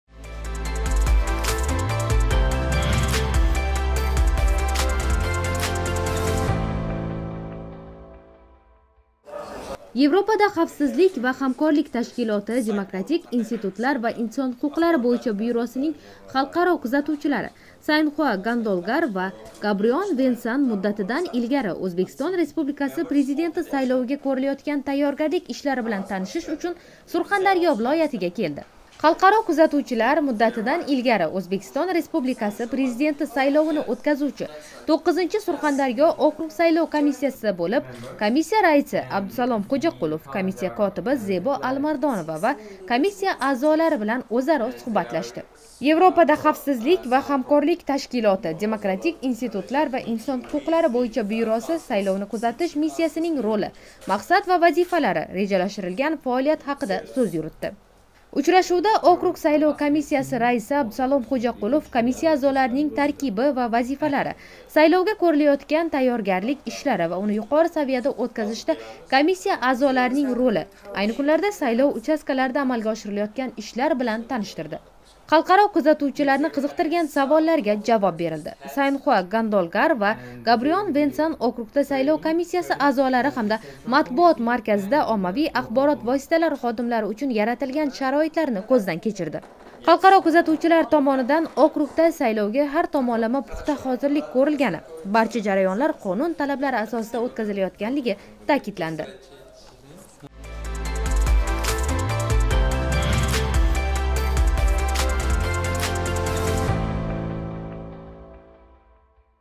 ХАЛҚАРО КУЗАТУВЧИЛАР ОКРУГ САЙЛОВ КОМИССИЯСИ ФАОЛИЯТИ БИЛАН ТАНИШМОҚДА
Европада хавфсизлик ва ҳамкорлик ташкилоти Демократик институтлар ва инсон ҳуқуқлари бўйича бюросининг халқаро кузатувчилари Сайнхуу Гандолгар (Монголия) ва Гадбильон Венсан (Франция) муддатидан илгари Ўзбекистон Республикаси Президенти сайловига кўрилаётган тайёргарлик ишлари билан танишиш учун Сурхондарё вилоятига келди.